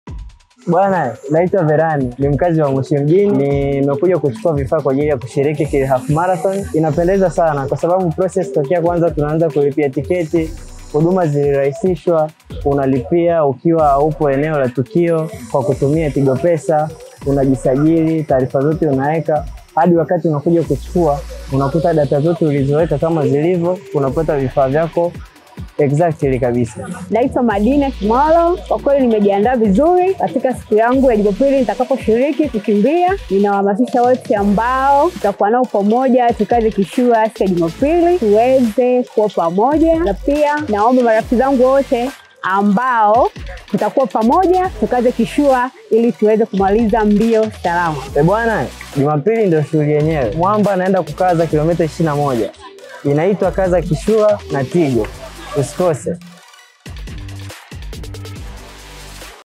UMEJIWEKA TAYARI? Kila Mtu yuko tayari kushiriki wewe je
UMEJIWEKA TAYARI? Kila Mtu yuko tayari kushiriki Tigo Kili Half Marathon 2023 na kuhakikisha wana-kaza kishua mpaka KM 21 zimamilike. Umejiandaaje?